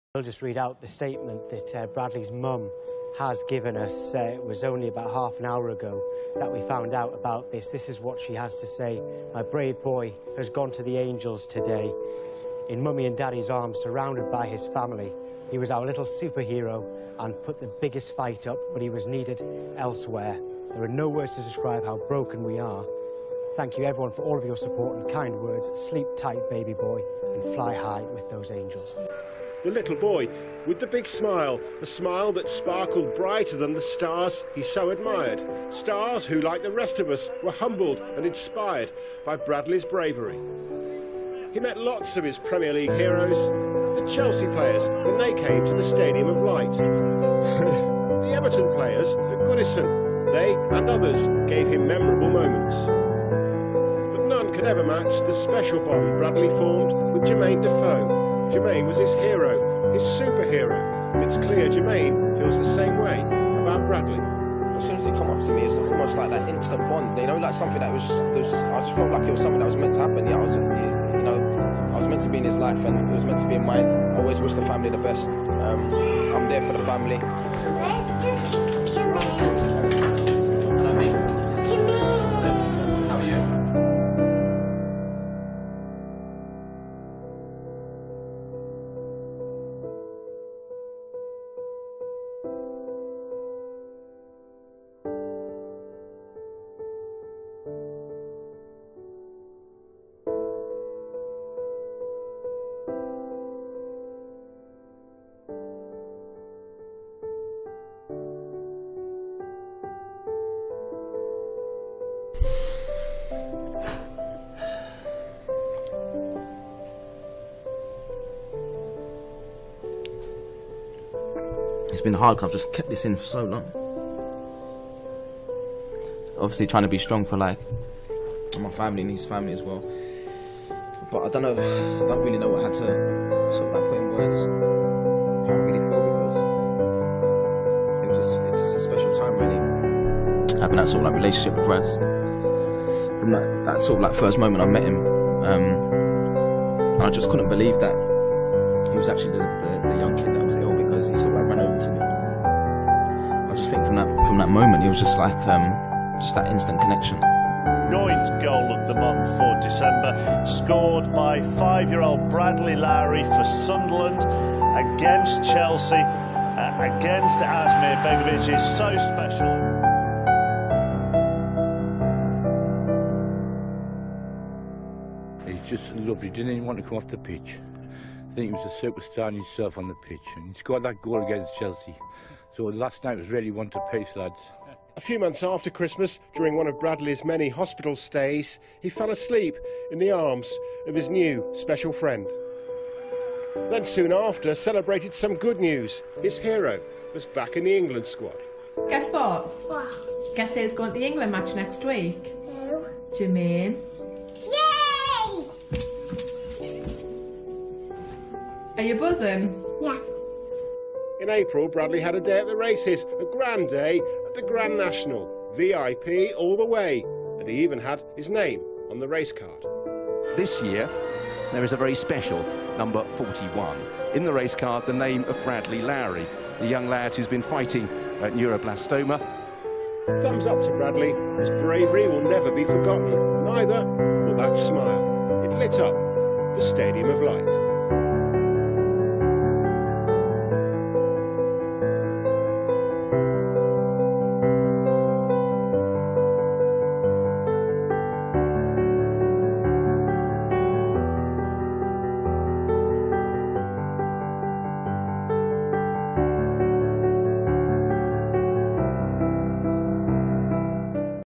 A brave little lad that none of us in Sunderland will ever forget. Play in heaven little Bradley.
Bradley Lowery is the little lad that captured the hearts of all Sunderland players, fans and football fans all over England and the world. He is the bravest little lad that I have ever met. He faced his illness always with a happy smile on his face and made everyone that met him smile. Thank you for making me smile and for making my struggles seem like nothing an inspiration to everyone and one of us, one of our own, one of the Lads. A true Sunderland legend, never will he be forgotten. Love you Bradley.